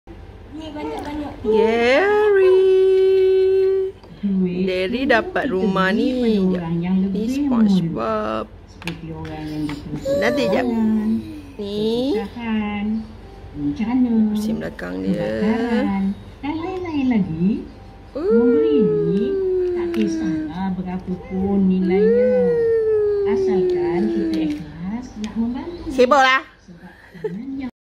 SpongeBob Squarepant Happy Meal!
Masa untuk berseronok dengan Krusty Krew! Kumpulkan koleksi Happy Meal dengan mainan "Gary & the Pineapple House" atau "SpongeBob & the Krusty Krab“ minggu ini.

PERCUMA dengan setiap pembelian Happy Meal! Pesan sekarang melalui McDelivery, Pandu Lalu atau Bawa Pulang.